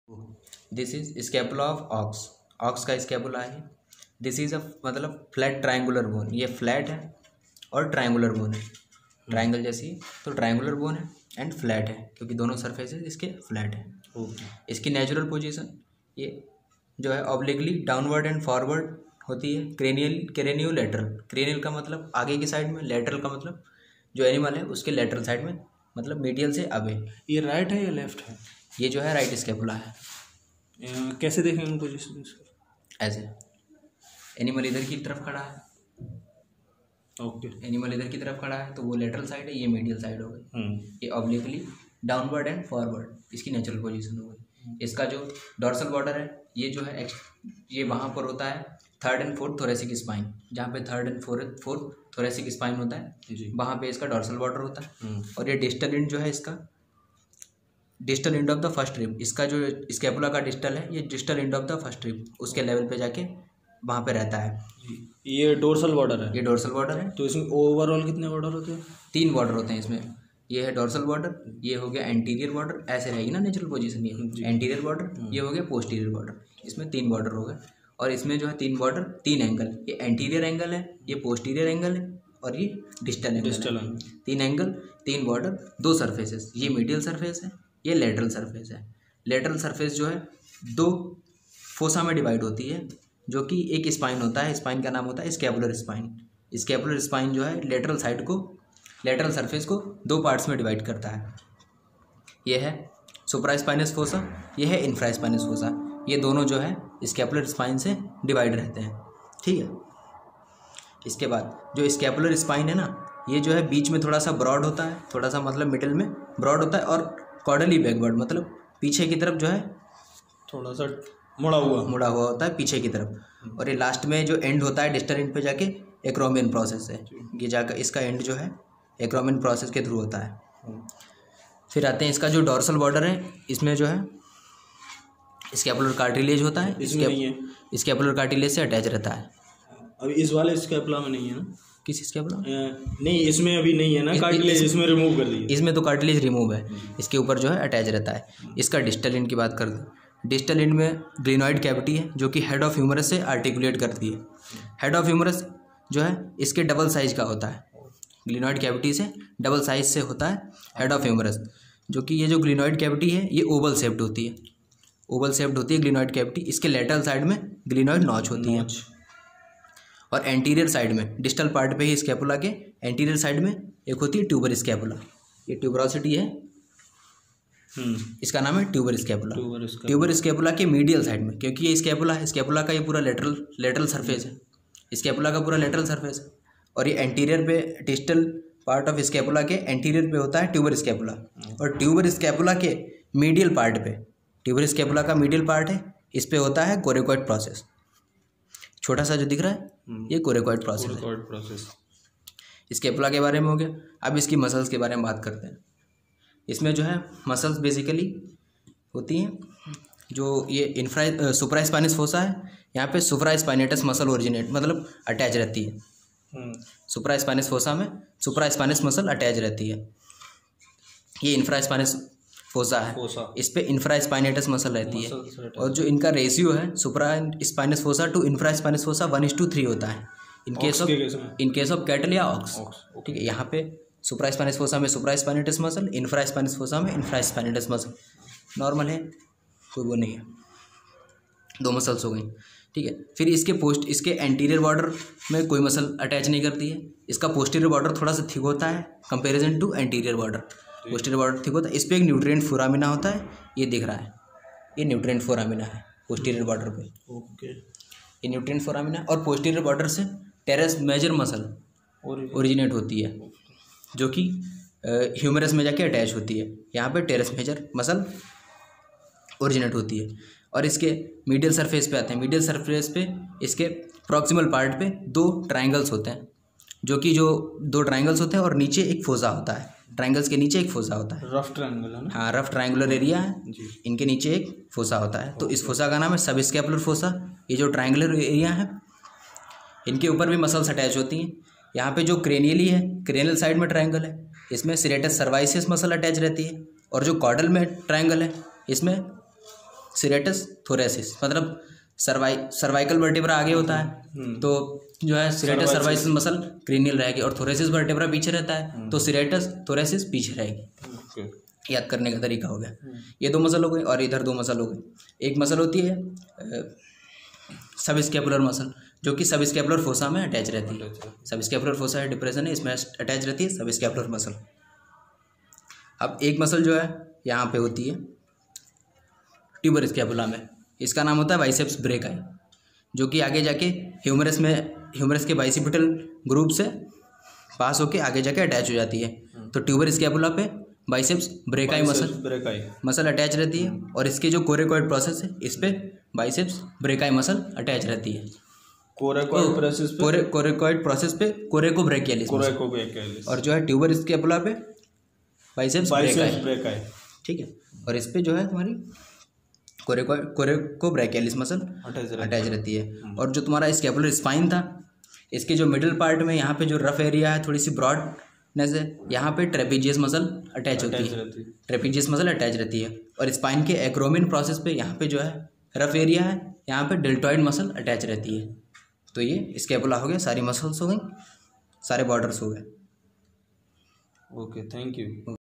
Detail Scapula of OX under bones of forelimb
Explanation of all surfaces, borders, muscles under scapula